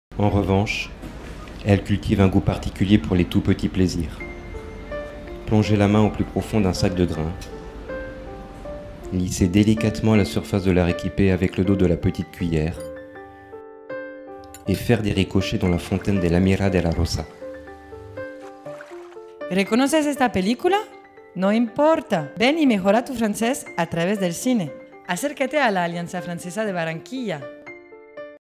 Promoción Curso de Francés del Cine - Alianza Francesa de Barranquilla (3)
Alianza Francesa de Barranquilla
Curso de Francés del Cine

Adaptación de Le fabuleux destin d'Amélie Poulain de Jean-Pierre Jeunet
Dirección y edción: Jhoy Suarez
Actuación: Emmanuelle Gines